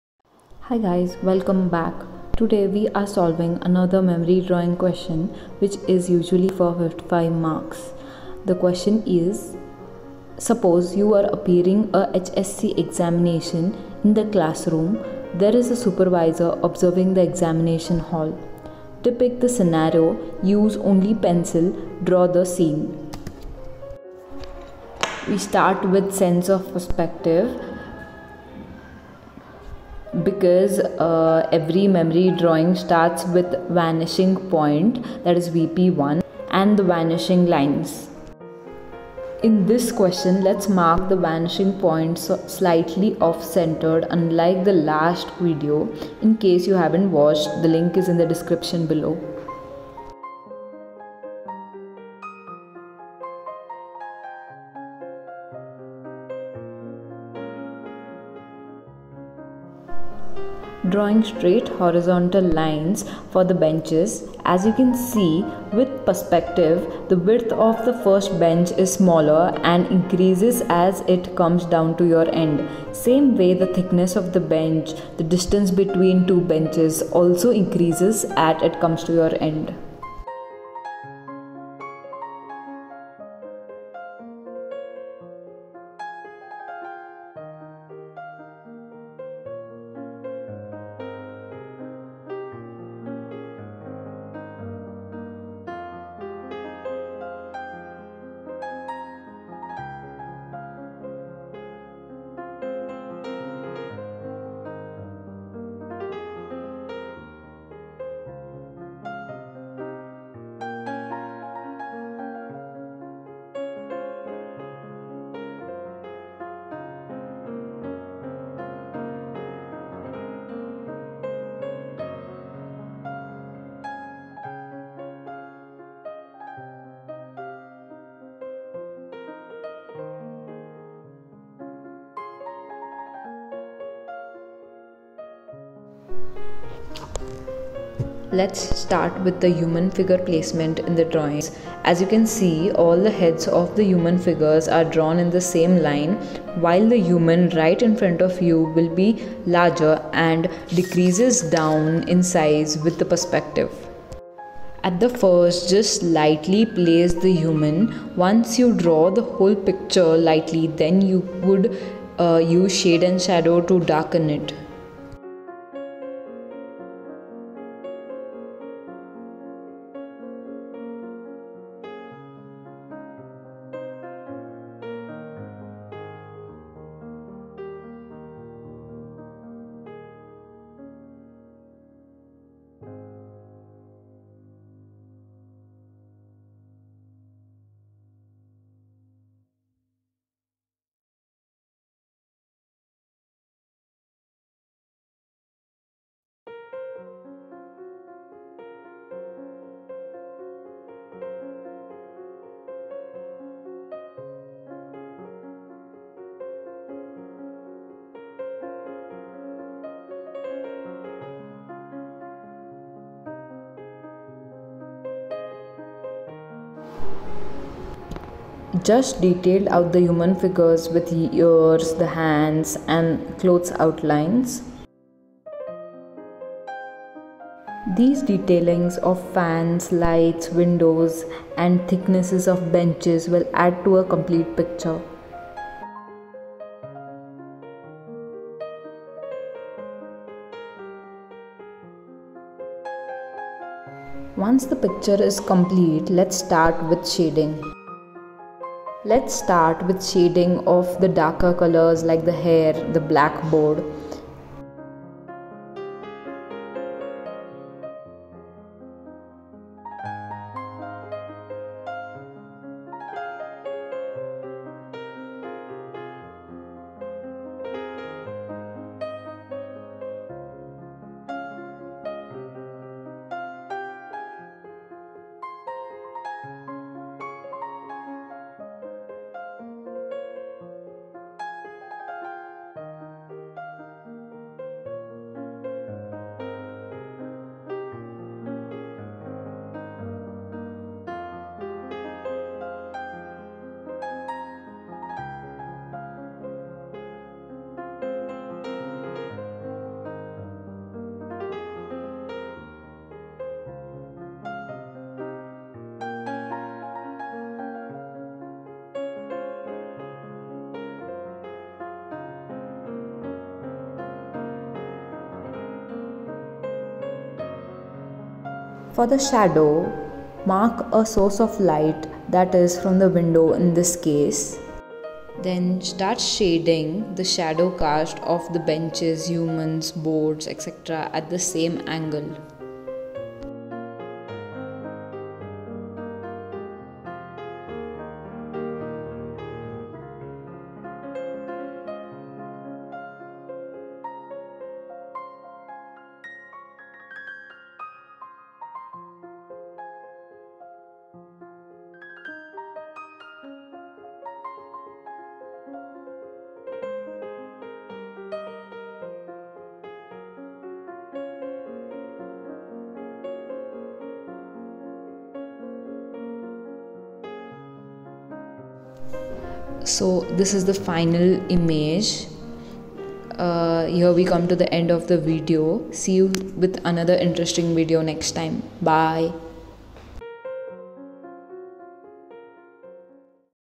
NATA/JEE-B.Arch Memory Drawing solved question |Perspective| Architecture Entrance Exam |Rough Book
This video solves Memory Drawing question for NATA Entrance Exam & JEEB.arch here the Detailed Drawing of the question is shown and also mentioned the marks {Architecture Entrance Exam}.all the guidelines on which the paper is judged are also mentioned